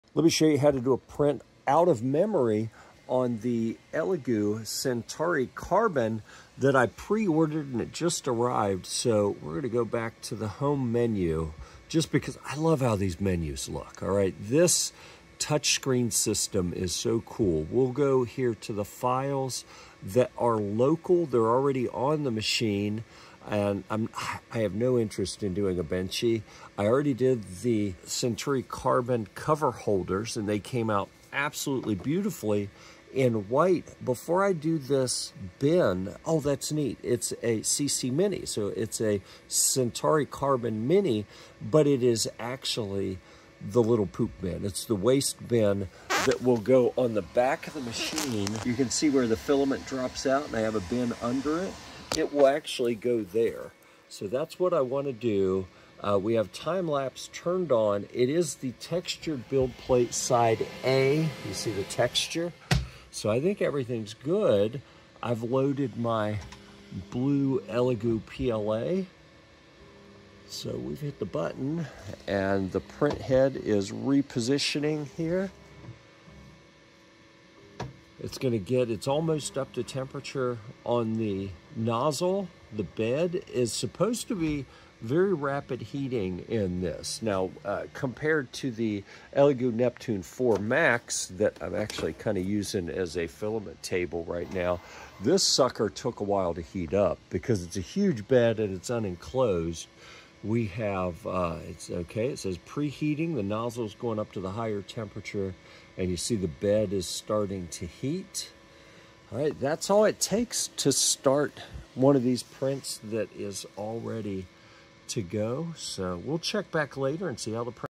Elegoo Centauri Carbon: First Print & Review
we demonstrate how to print a memory file on the Elegoo Centauri Carbon, showcasing its touchscreen system, fast heating bed, and textured build plate. Watch our first print of the CC mini waste bin!Our Printers:

- SUNLU Official Filament Dryer Box S4,Four-Spool 3D Printer Dehydrator with Rapid Heating, Auto Humidity Control, and Advanced Triple Fan

Video tools
Studio mic
Livestream PC
4k gimble
Wireless Mic